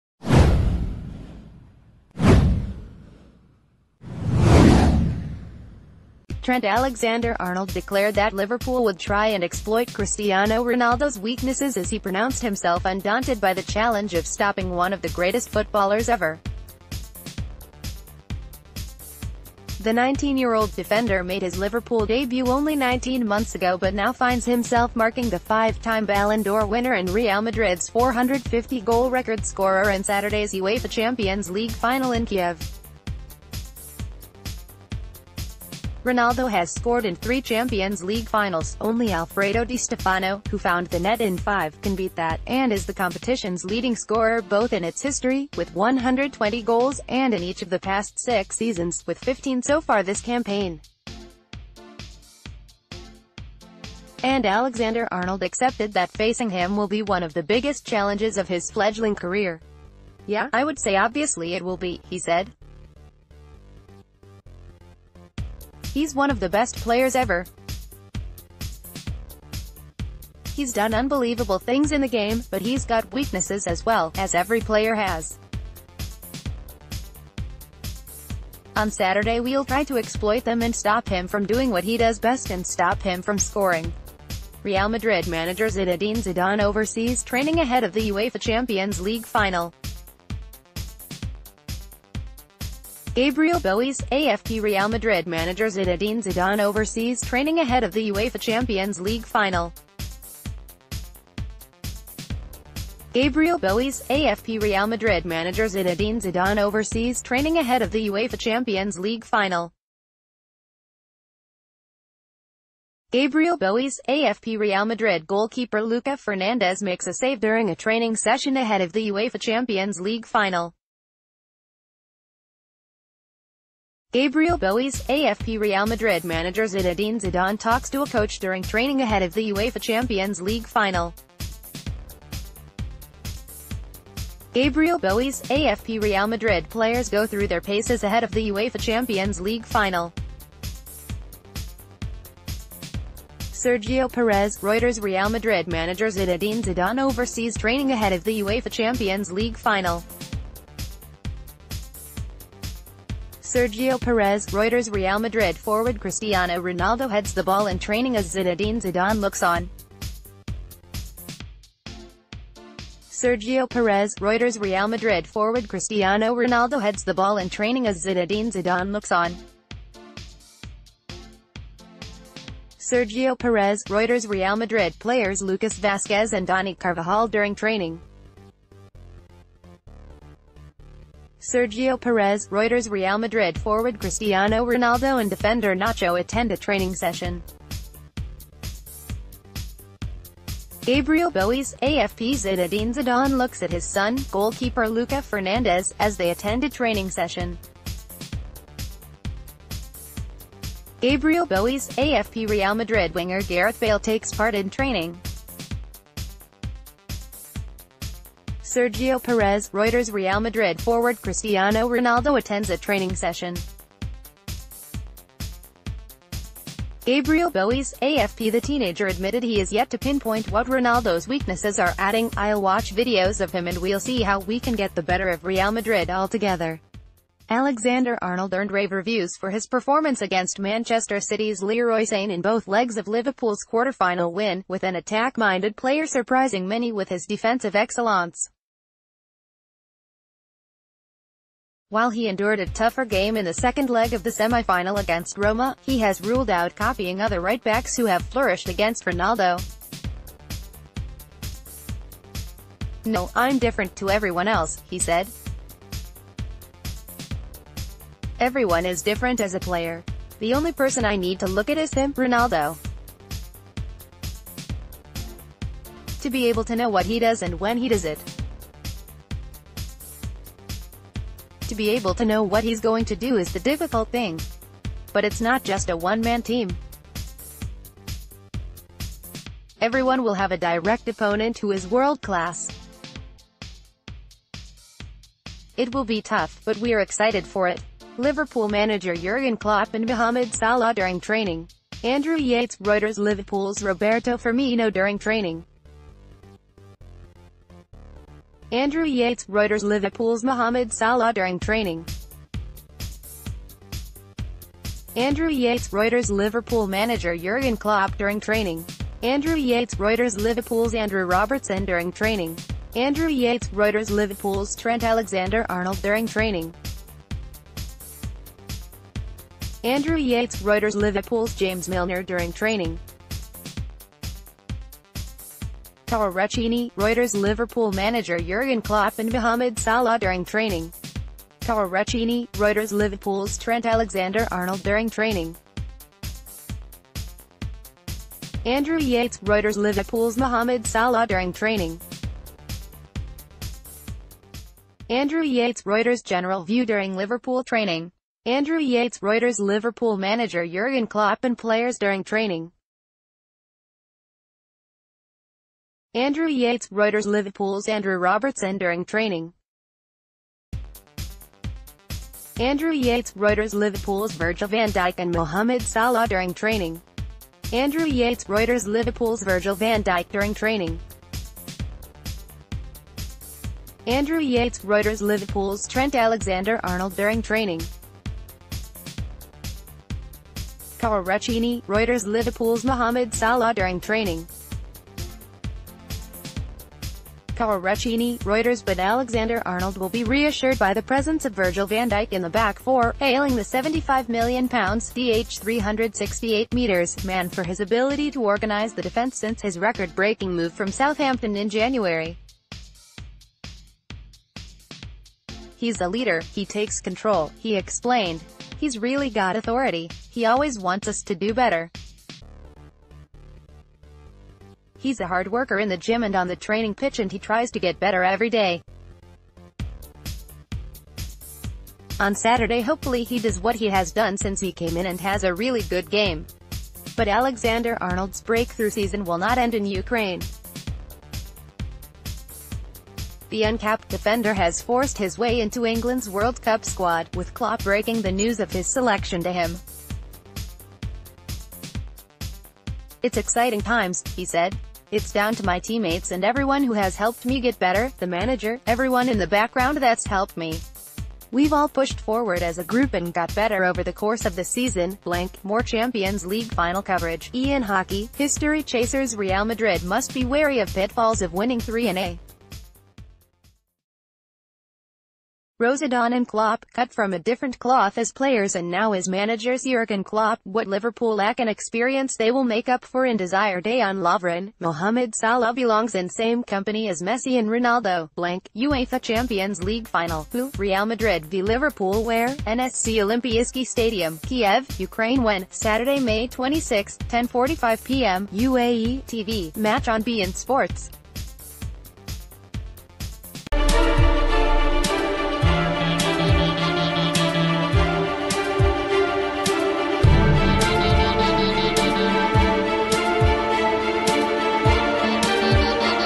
Champions League final: Alexander-Arnold up to the challenge of trying to stop 'one of the best p...
Champions League final: Alexander-Arnold up to the challenge of trying to stop 'one of the best players ever' in Ronaldo

Trent Alexander-Arnold declared that Liverpool would try and exploit Cristiano Ronaldo’s weaknesses as he pronounced himself undaunted by the challenge of stopping one of the greatest footballers ever. The 19-year-old defender made his Liverpool debut only 19 months ago but now finds himself marking the five-time Ballon d’Or winner and Real Madrid’s 450-goal record scorer in Saturday’s Uefa Champions League final in Kiev. Ronaldo has scored in three Champions League finals – only Alfredo di Stef...